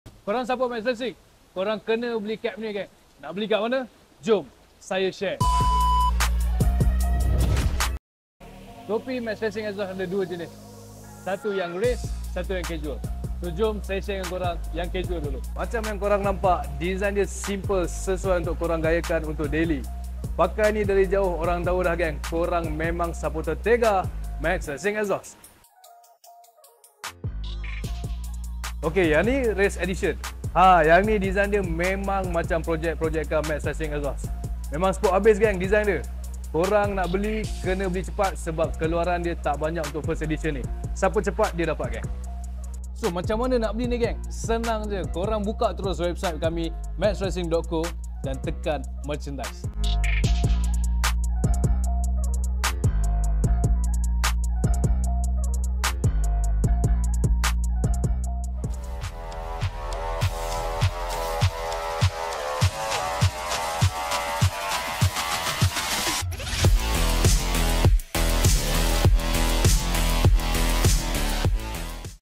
Max Racing Exhaust Official Racing Cap
More Than a Cap, It's a Statement of Performance.

This Max Racing Exhaust Black Racing Cap transcends mere headwear. It's a declaration of your unwavering dedication to peak performance and a love for the thrill of the race. Whether you're a driver pushing limits, a champion in the making, or a passionate fan, this cap embodies the spirit of Max Racing Exhaust.

By choosing the official Max Racing Exhaust Racing Cap, you're not just acquiring a stylish accessory; you're aligning yourself with a brand synonymous with pushing the boundaries of automotive performance.

Designs:
Iconic Yellow Edition